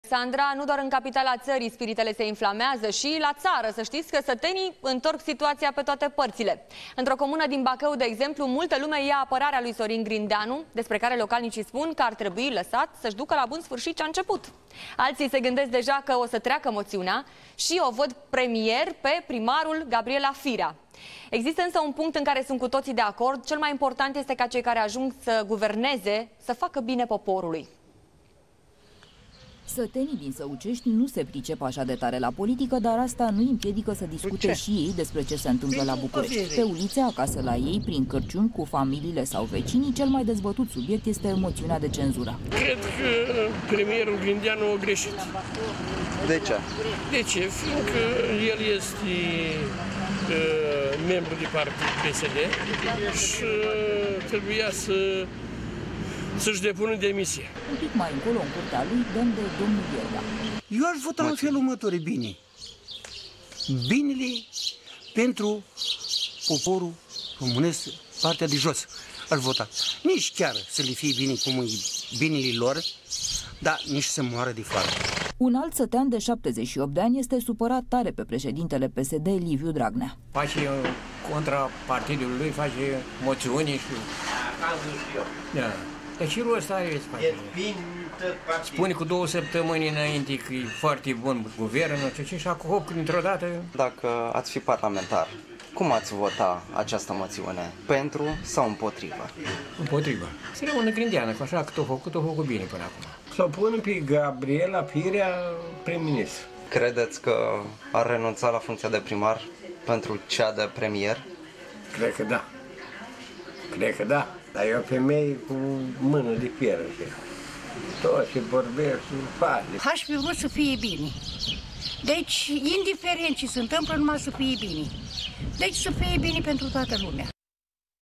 Moţiunea, dezbătută şi la sat
Vezi jurnalul de știri pe Antena Play Nu doar în Capitala ţării, spiritele se inflamează. Şi la ţară, sătenii întorc situaţia pe toate părţile. Într-o comună din Bacău, de exemplu, multă lume îi ia apărarea lui Sorin Grindeanu, despre care localnicii spun că ar trebui lăsat să ducă la bun sfârşit ce a început.